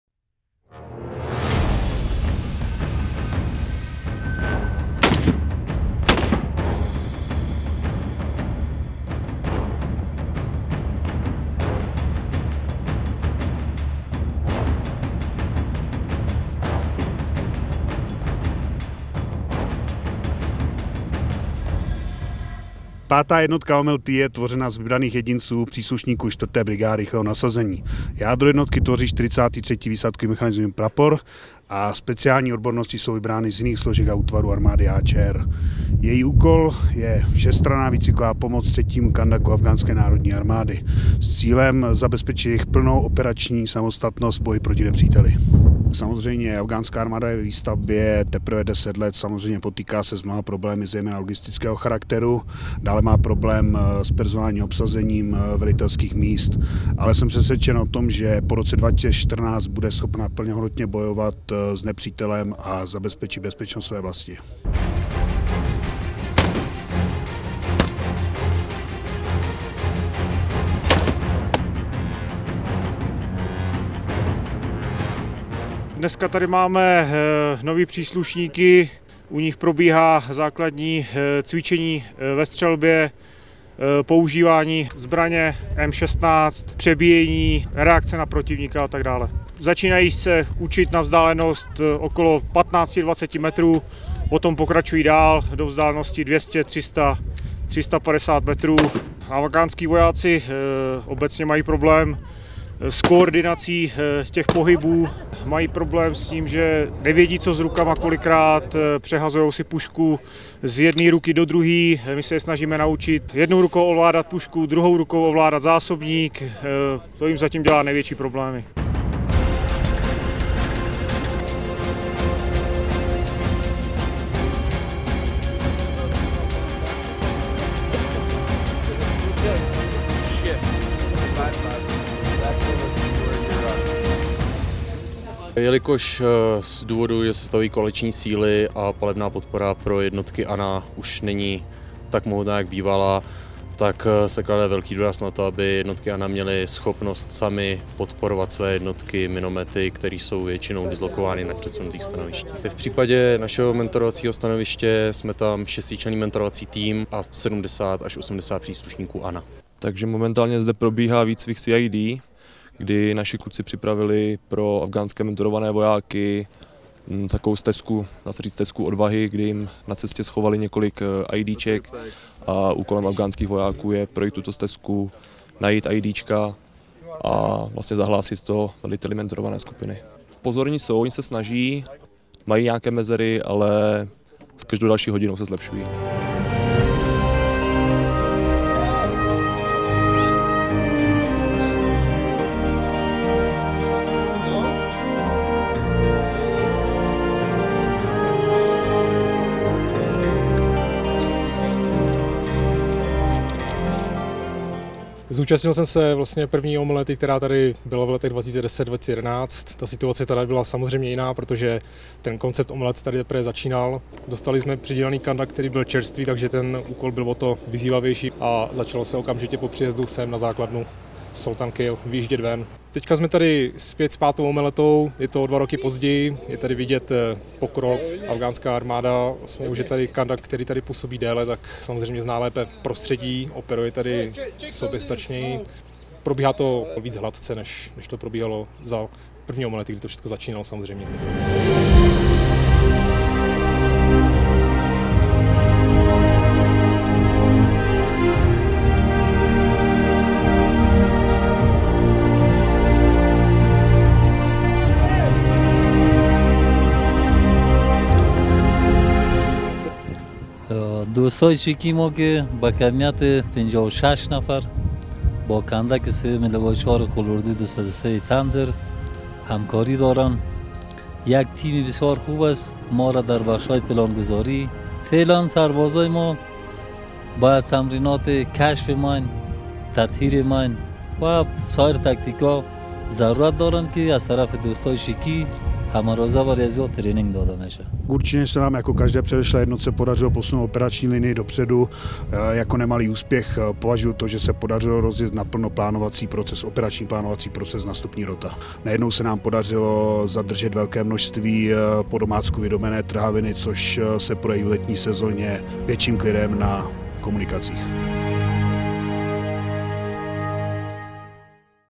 5. OMLT - mise poslední české "omelety" v rámci ISAF se chýlí ke konci
Tým složený převážně z chrudimských výsadkářů se chopil práce v provincii Wardak v polovině října minulého roku. Jeho mise se tak chýlí ke konci. Stal se posledním v řadě českých „omelet". Teď mu zbývá předat základnu, materiál a zkušenosti týmu MAT.

„Určitě se nám podařilo posunout operační linii dopředu. Jako nemalý úspěch vnímám rozjezd plánovacího procesu na stupni afghánské roty. Zadrželi jsme také značné množství podomácku vyráběných trhavin, což se příznivě projeví v letních měsících větší bezpečností na místních komunikacích," shrnuje půlroční práci velitel 5. jednotky OMLT major Igor Jašek.